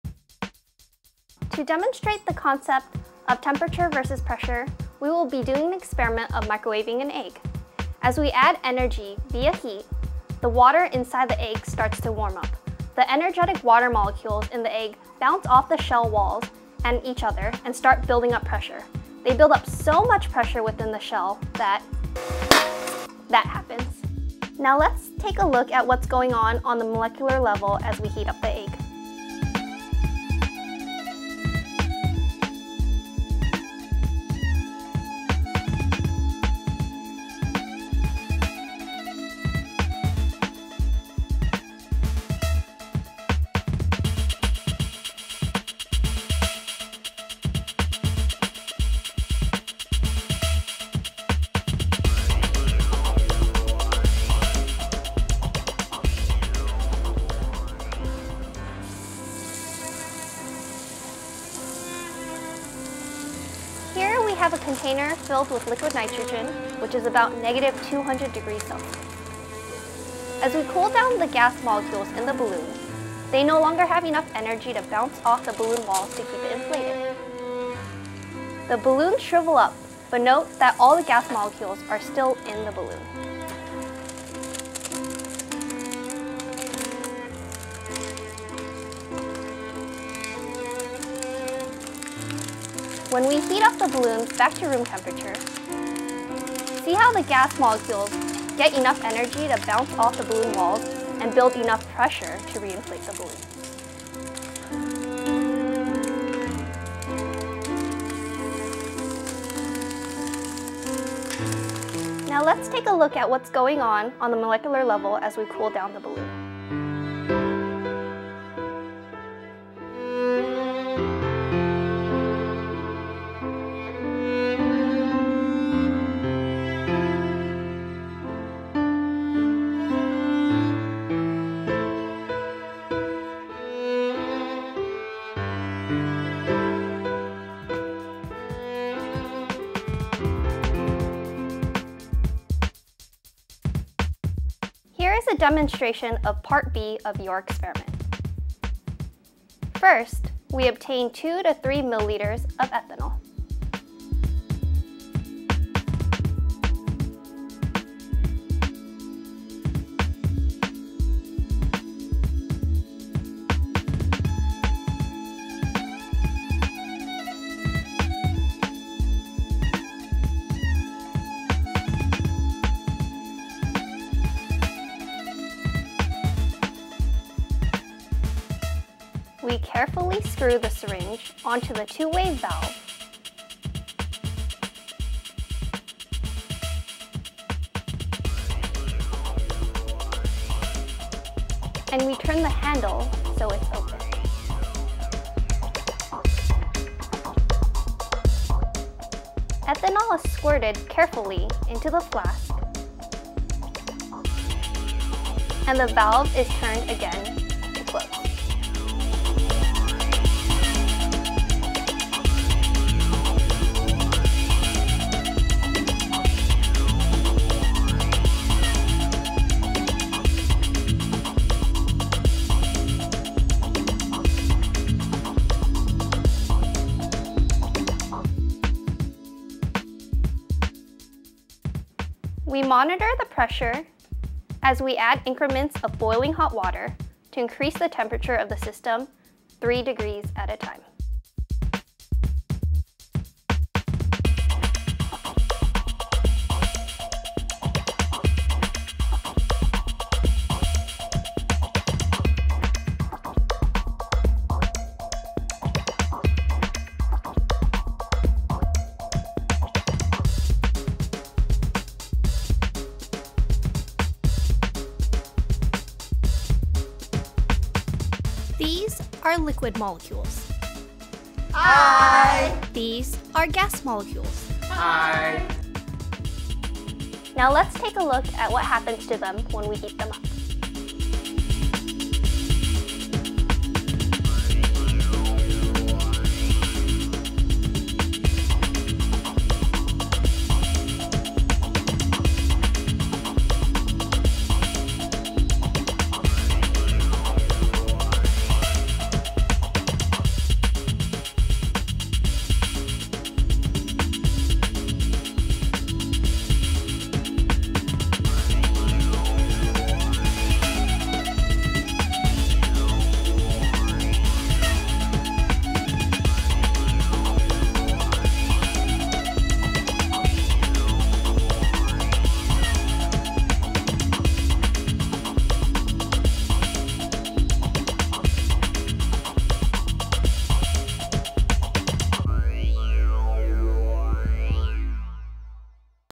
Temperature vs. Pressure
With the use of dancers, this video shows the relationship between temperature and pressure. Watch the "molecules" heat up and cool down.